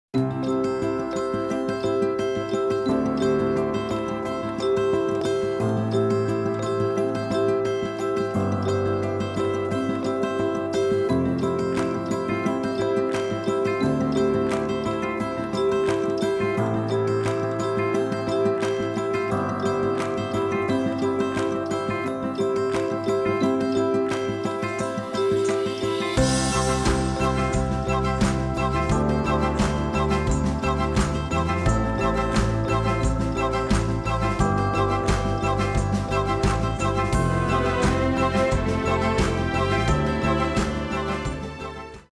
N Hayat... Bir ayda koşacak kadar iyileşti
Uludağ Üniversitesi Veteriner Fakültesi öğrencileri, bacakları eğri doğduğu için ayağa kalkamayan oğlağı bir ayda koşacak hale getirdi.